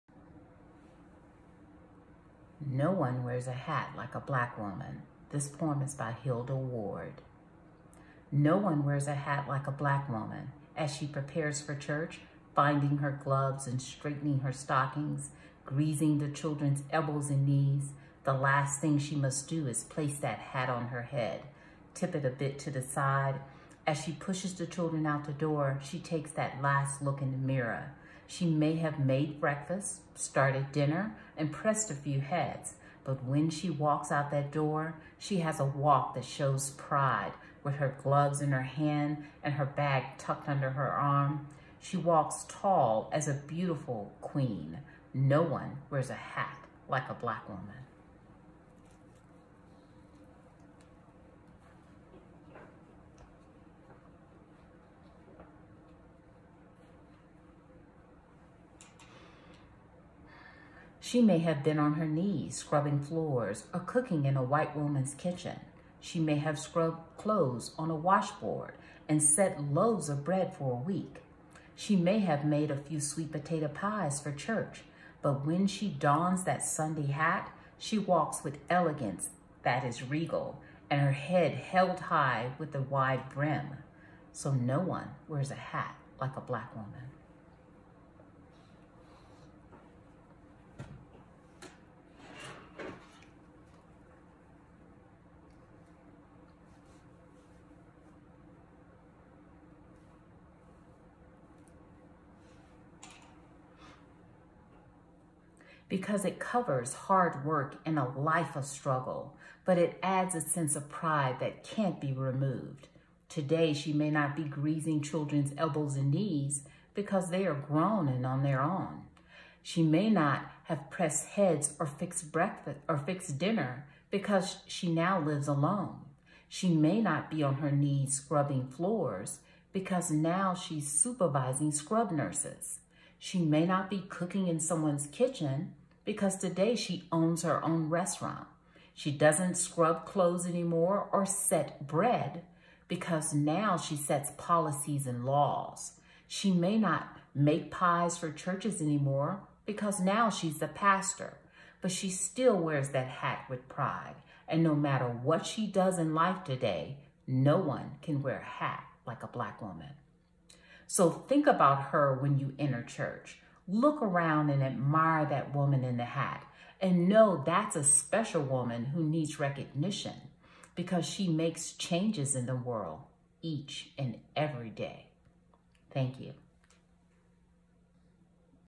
The Crown Video
Wonderful recitation of a poem penned by Hilda Ward "No One Can Wear a Hat Like a Black Woman" by Sonya Aldridge.